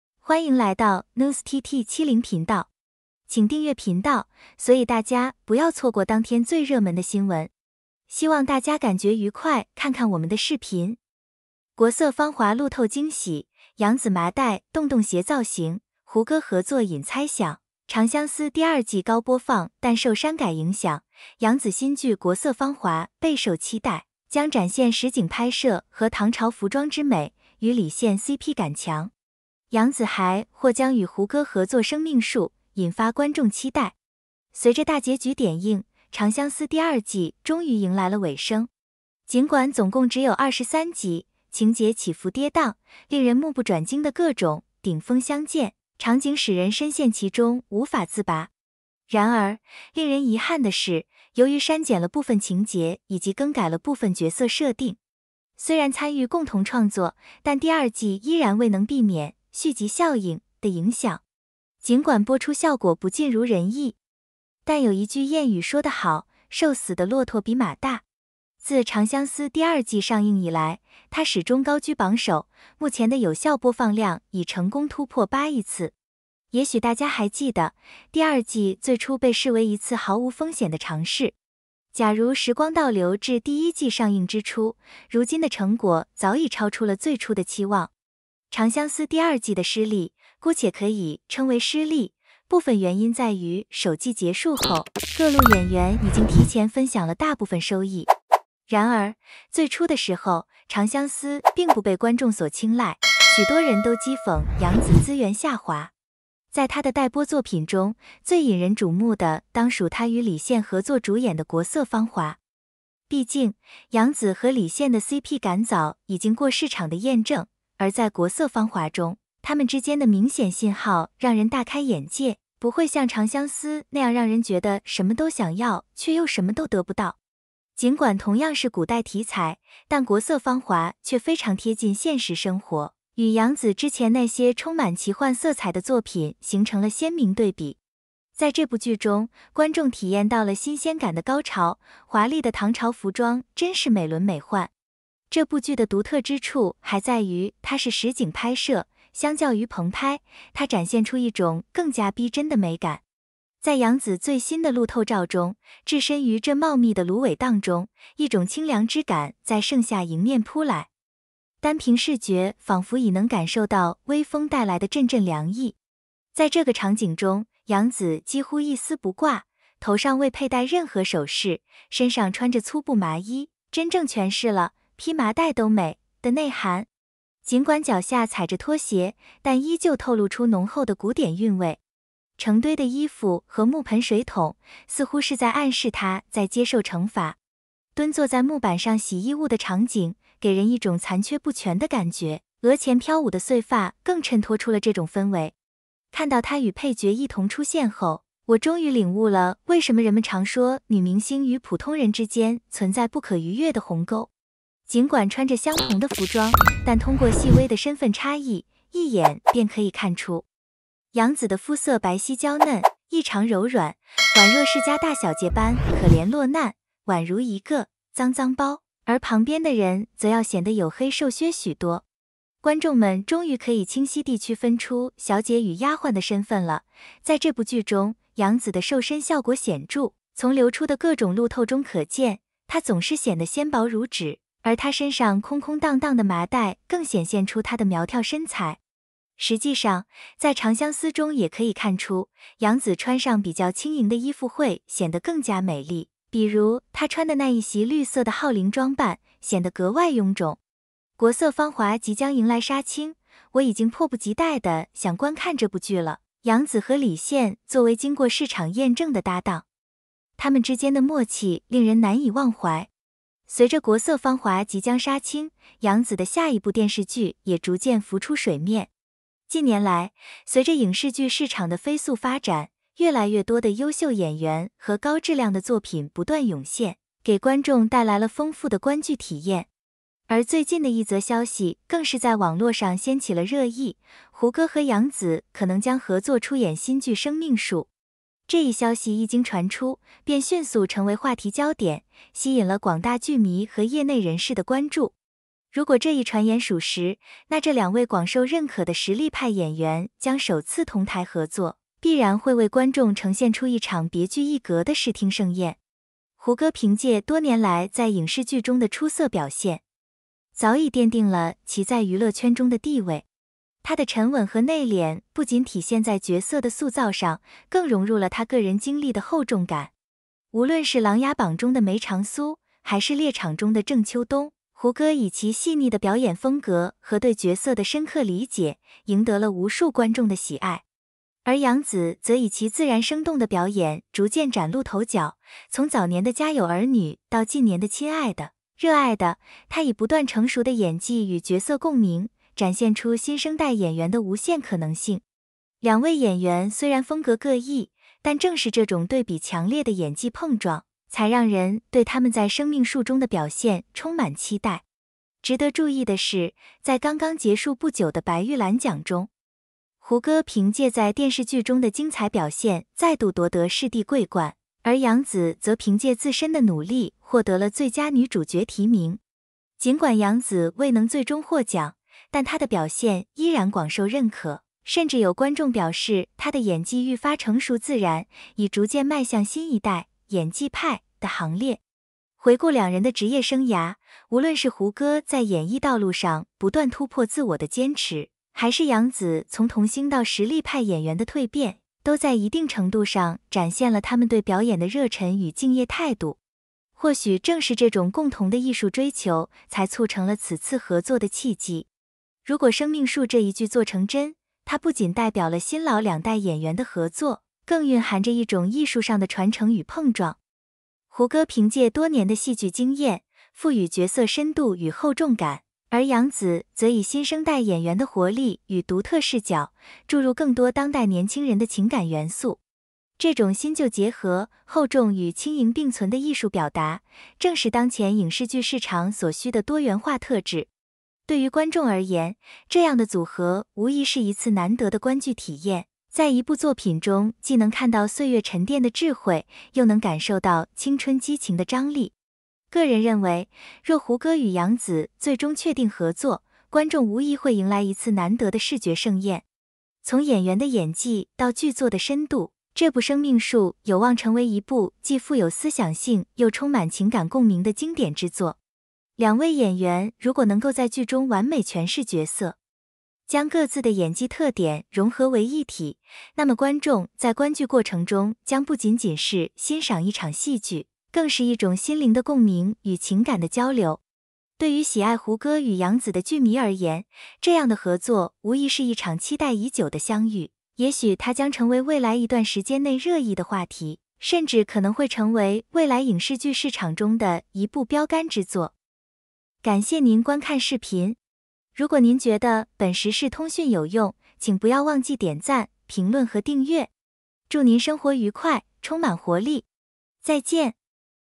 国色芳华剧照出炉！杨紫 奇特造型引发热议，胡歌 再度合作成焦点！yangzi
《国色芳华》即将迎来杀青，我已经迫不及待地想观看这部剧了，杨紫和李现作为经过市场验证的搭档，他们之间的默契令人难以忘怀。
随着《国色芳华》即将杀青，杨紫的下一部电视剧也逐渐浮出水面。
近年来，随着影视剧市场的飞速发展，越来越多的优秀演员和高质量的作品不断涌现，给观众带来了丰富的观剧体验。而最近的一则消息更是在网络上掀起了热议——胡歌和杨紫可能将合作出演新剧《生命树》。这一消息一经传出，便迅速成为话题焦点，吸引了广大剧迷和业内人士的关注。如果这一传言属实，那这两位广受认可的实力派演员将首次同台合作，必然会为观众呈现出一场别具一格的视听盛宴。
胡歌凭借多年来在影视剧中的出色表现，早已奠定了其在娱乐圈中的地位。他的沉稳和内敛不仅体现在角色的塑造上，更融入了他个人经历的厚重感